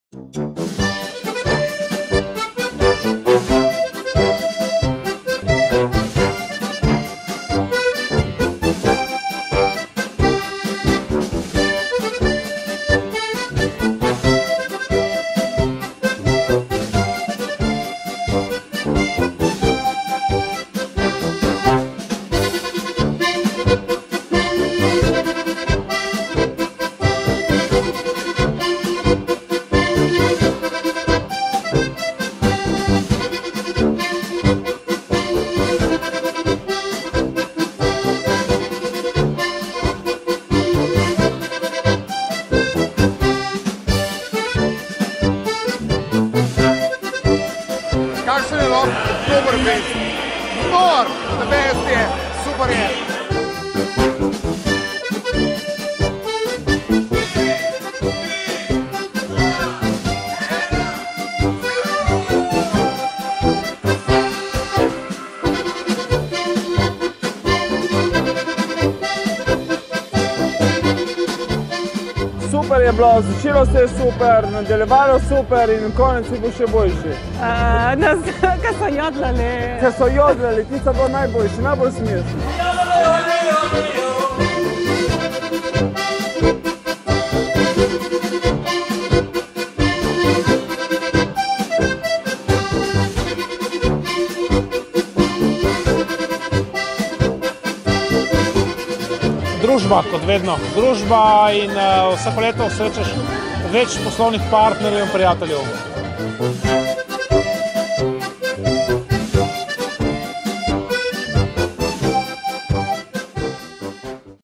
Oktoberfest Porsche Ljubljana 2016
Pravo bavarsko doživetje na slovenskem Oktoberfestu Porsche Ljubljana 2016. Skozi večer nas je tokrat zabaval voditelj in igralec Jože Robežnik. Za glasbo so poskrbeli ansambel Slovenski zvoki in Mengeška godba. Tokrat pa je bila slovenska premiera novega Volkswagna Amaroka, ki je bil pripeljan direktno z mednarodnega avtomobilskega salona v (IAA) v Hannovru. Skupaj z nami so ga predstavil Franci Petek in nadobudni mladi skakalci. Razvajali pa smo se tudi s pravimi bavarskimi specialitetami ter točili pivo Paulaner.